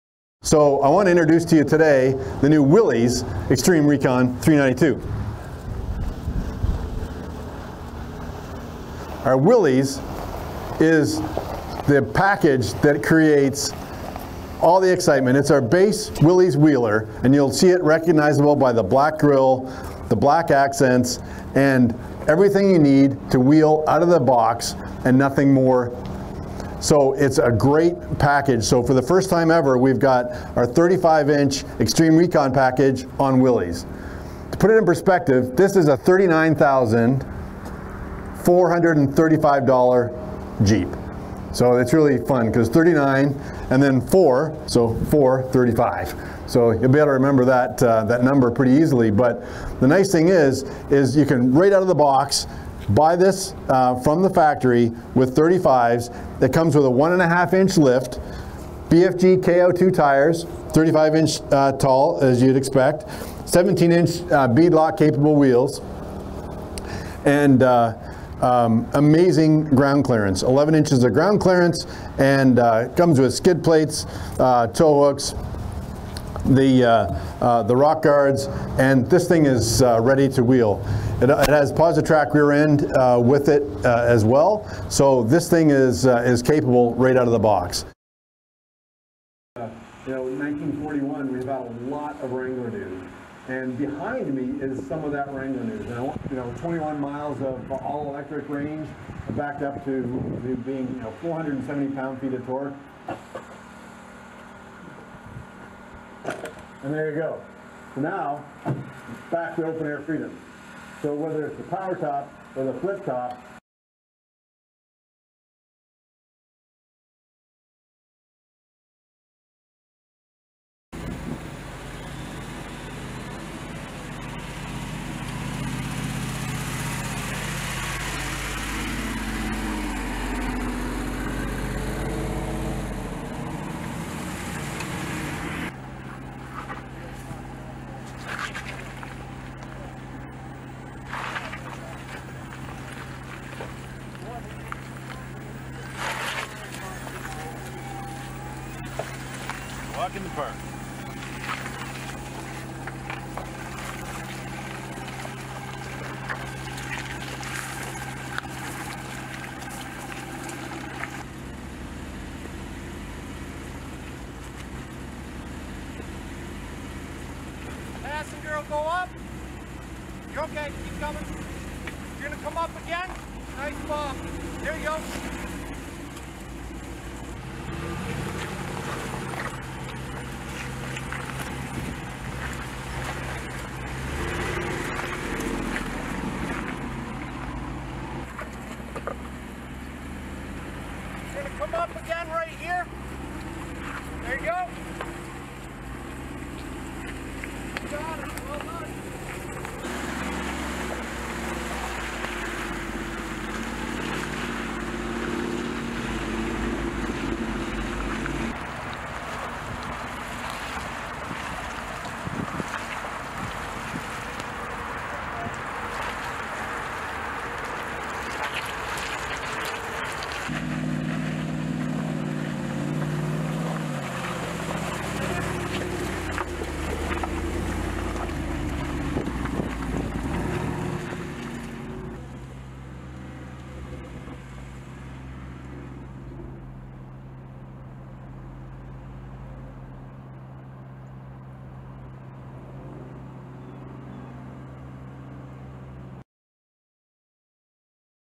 2022 JEEP Wrangler Willys With Xtreme Recon Package | Shows its OFF-ROAD Skills
Take a look at the New 2022 JEEP Wrangler Willys With Xtreme Recon Package. You can see in this video how this Wrangler is doing off-road.

    Jeep® Wrangler Willys with Xtreme Recon Package offers 35-inch tires straight from the factory
    Equipped with the $3,995 Xtreme Recon Package, Jeep Wrangler Willys joins Wrangler Rubicon and Wrangler Rubicon 392 to deliver best-in-class approach angle, departure angle, ground clearance and water fording capability
    Customer orders open in October for Wrangler Willys with Xtreme Recon Package, with production starting at the Toledo (Ohio) Assembly Plant in December

Priced at a U.S. manufacturer’s suggested retail price (MSRP) of $39,435 (plus $1,495 destination charge), the 2022 Jeep Wrangler Willys with the Xtreme Recon Package includes:

    LT315/70R17C (35-inch) BF Goodrich KO2 all-terrain tires
    17-inch by 8-inch beadlock capable wheels
    Swing gate reinforcement
    4.56:1 axle ratio
    1.5-inch factory suspension lift with uniquely tuned shocks
    Anti-lock four-wheel-disc performance brakes

Fitted with a standard black grille, matte black “Jeep” badge, black “Trail Rated” badge, “4-Wheel Drive” swing gate decal and “Willys” hood decal, the heritage-inspired Wrangler Willys brings a unique appearance to the Wrangler lineup, bolstered for 2022 with the available Xtreme Recon 35-inch tire package.

“The new Jeep Wrangler Willys with Xtreme Recon Package gives our passionate customers another great option for straight-from-the-factory 35-inch tires,” said Jim Morrison, Vice President, Jeep Brand North America. “Named after the original Jeep utility vehicle, the Willys holds special meaning to the Jeep brand, and we’re excited to enhance the new Wrangler Willys with 35-inch BF Goodrich KO2 tires.”

Equipped with the Xtreme Recon Package, the Wrangler Willys delivers best-in-class approach angle, departure angle, ground clearance and water fording capability:

    Approach angle – 47.4 degrees
    Breakover angle – 26.7 degrees
    Departure angle – 40.4 degrees
    Ground clearance – 12.9 inches
    Water fording – 33.6 inches

Customers can order the Xtreme Recon Package on the Jeep Wrangler Willys 3.6L eTorque starting in October. Production at the Toledo (Ohio) Assembly Plant will start in December.

Source : Jeep